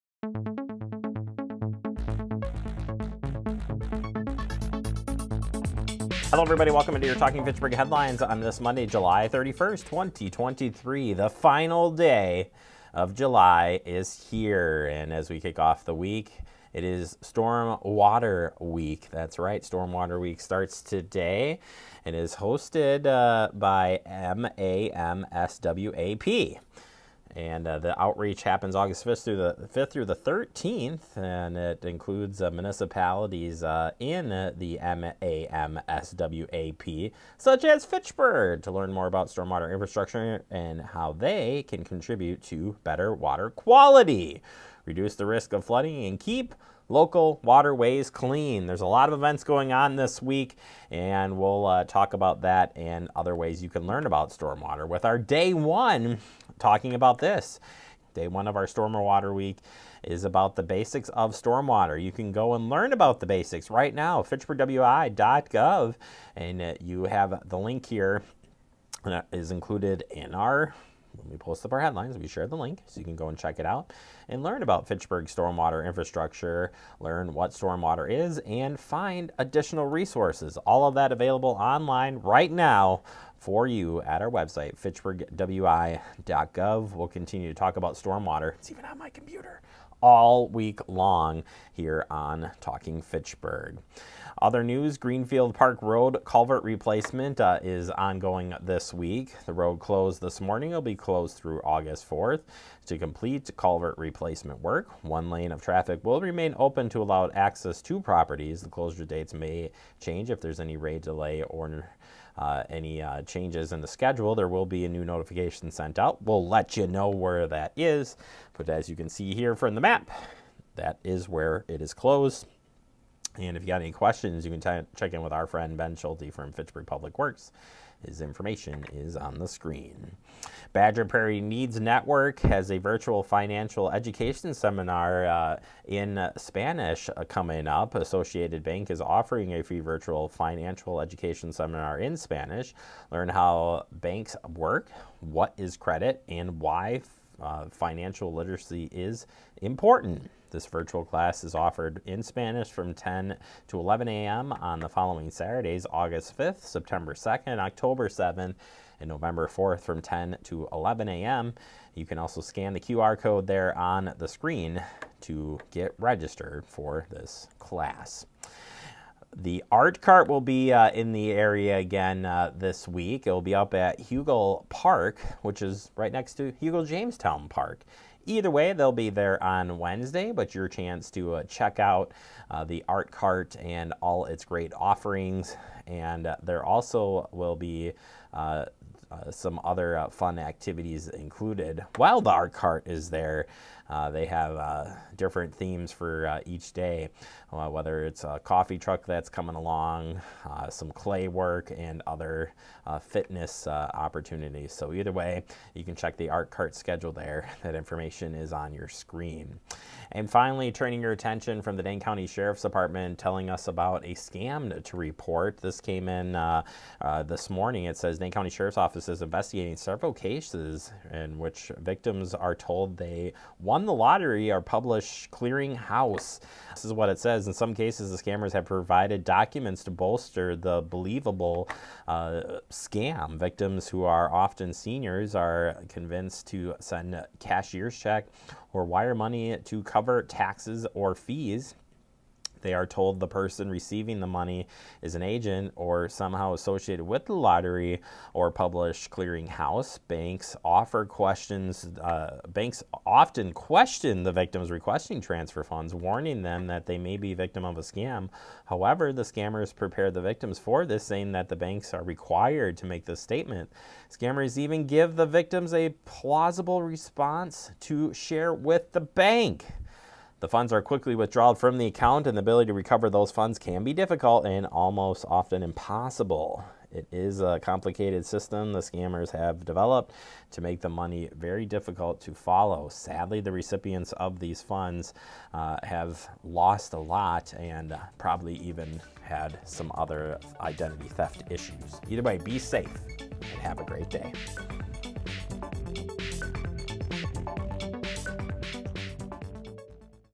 Talking Fitchburg Daily Headlines 7-31-23
Here are your Talking Fitchburg headlines for July 31st!
Watch the video to learn more or check out the links to our stories below…

Stormwater Week Starts August 5th!